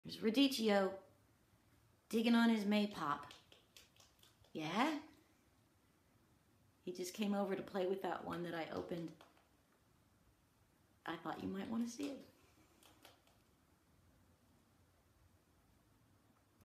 Radicchio digs on his maypop
Small Pet Select foster rabbit Radicchio is totally digging this maypop.  Thanks to Rabbit Wranglers for letting us care for him - he's a dream!